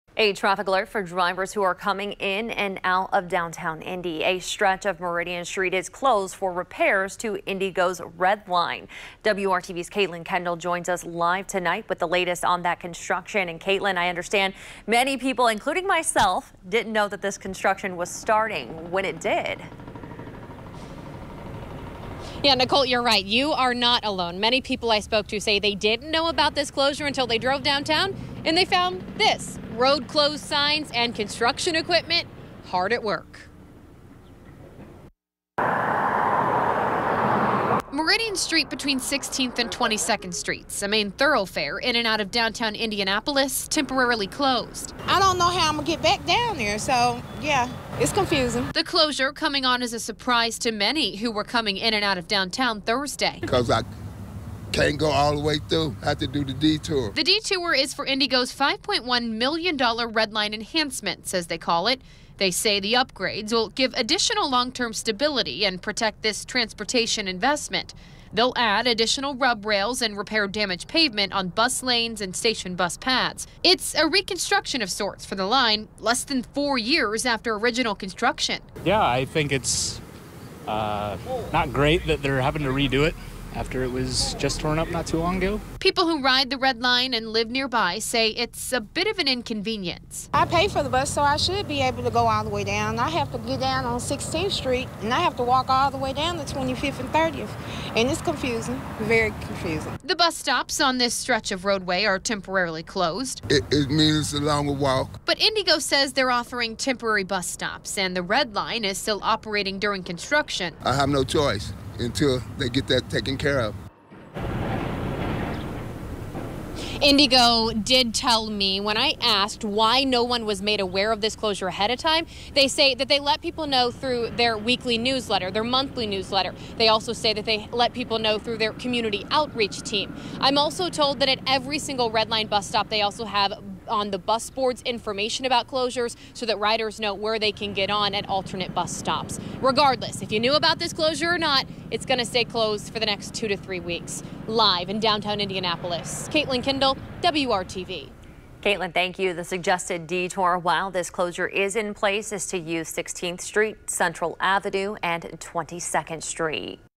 IndyGo Red Line enhancements close Meridian Street
You may have noticed a significant closure Thursday morning as you drove into downtown Indy from the north.